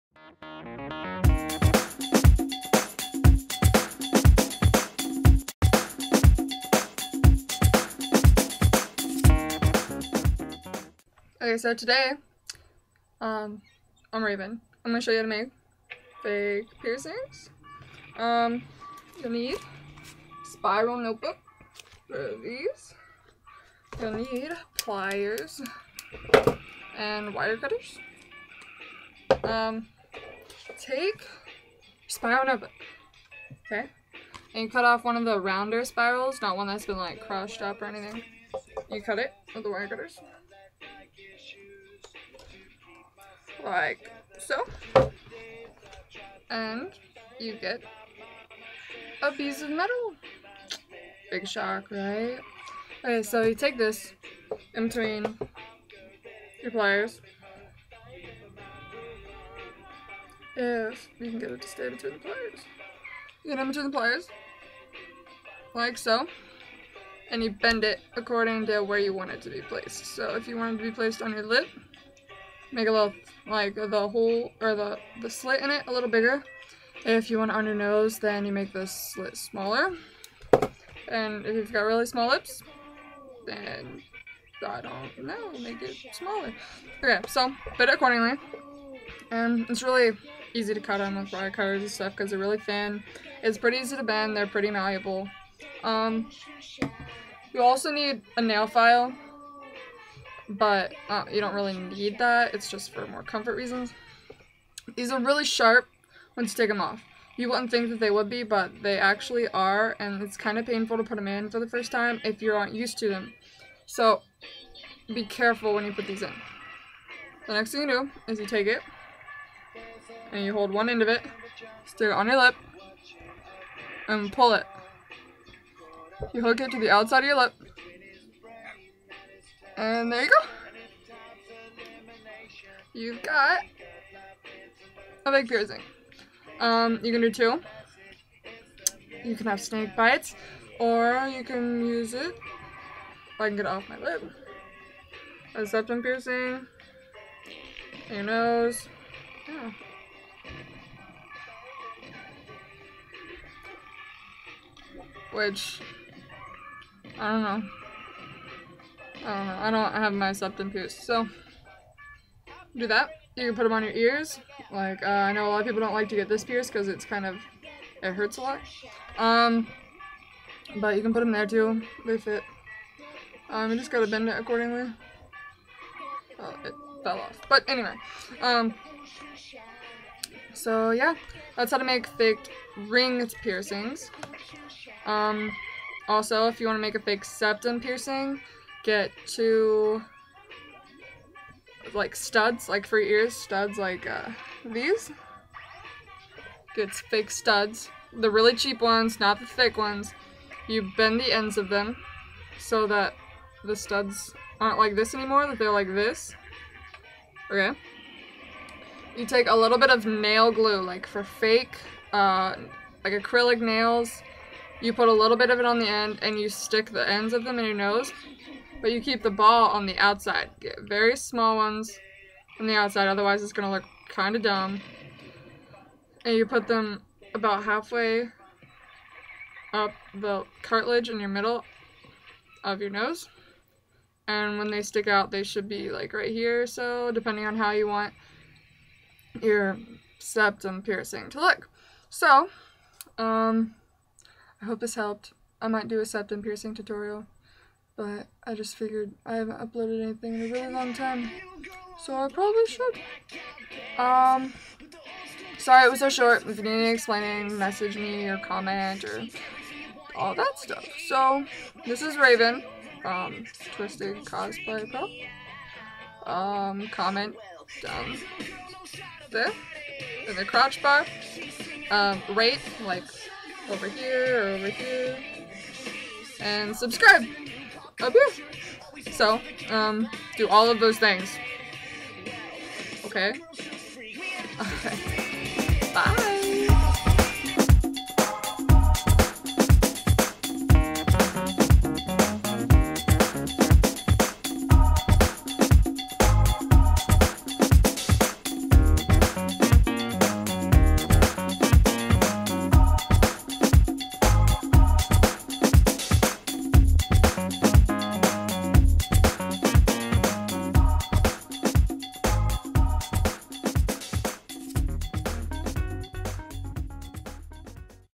FAKE PIERCING TUTORIAL
i havent uploaded a video in forever, so i decided to do this one for all my little cosplayers and cosplayettes, or you know. normal people... lol, anyway, hope you like it:)